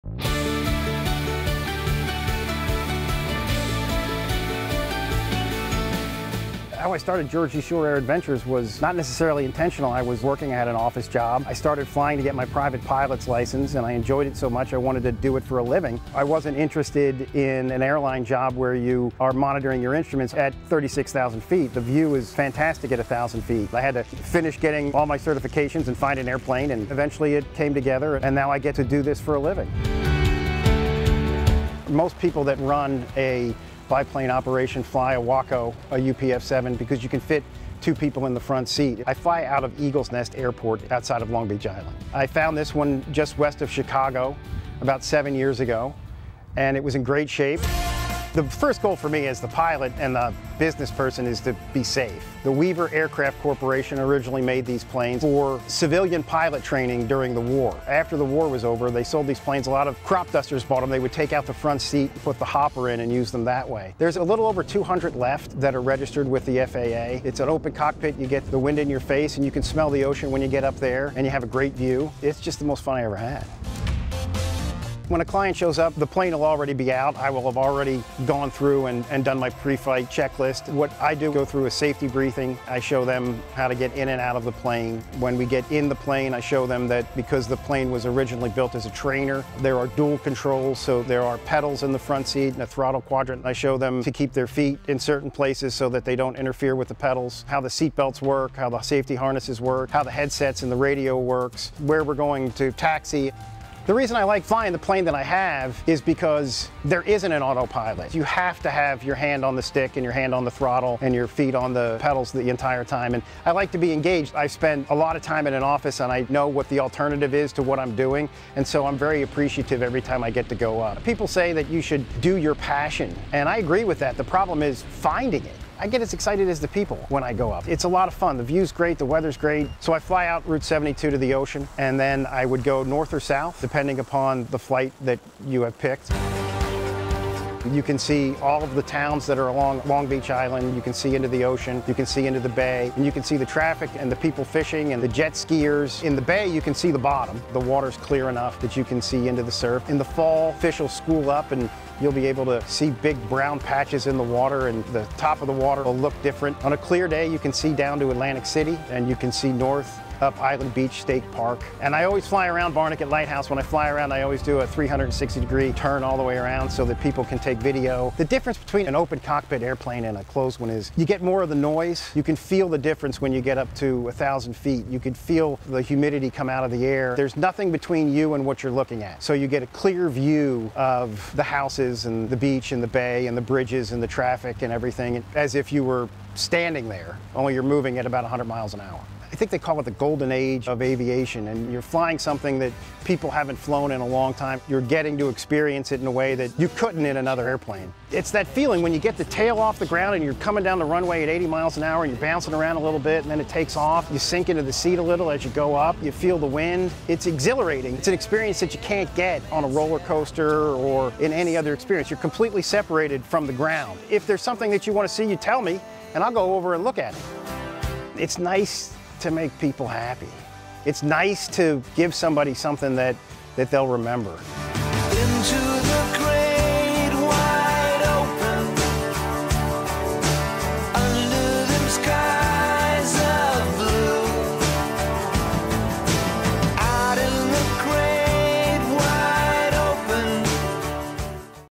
JERSEY SHORE AIR ADVENTURES
SOME SAY THAT IF YOU FIND A JOB THAT IS ALSO YOUR PASSION, YOU'LL NEVER TRULY "WORK" A DAY IN YOUR LIFE.   NEW JERSEY RESIDENT, GUY CORRADINO DID JUST THAT, LEAVING HIS HIGH LEVEL OFFICE JOB BEHIND AND TURNING HIS "HOBBY" INTO A HIGH FLYING CAREER IN THE SKIES WITH JERSEY SHORE AIR ADVENTURES!!